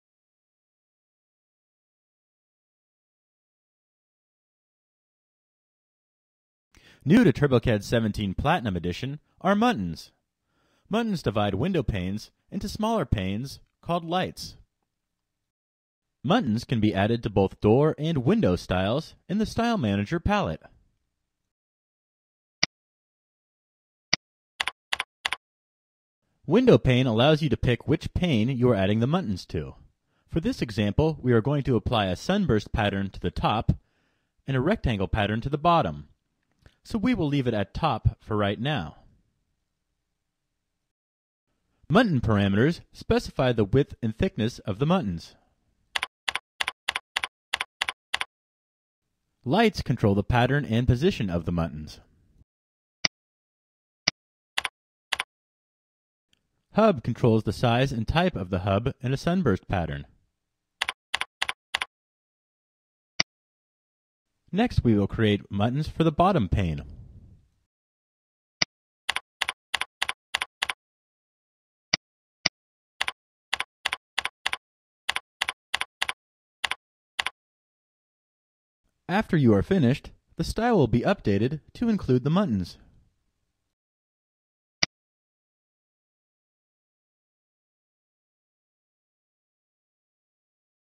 Muntins
New in TurboCAD 17 is the ability to add muntins to door and window styles.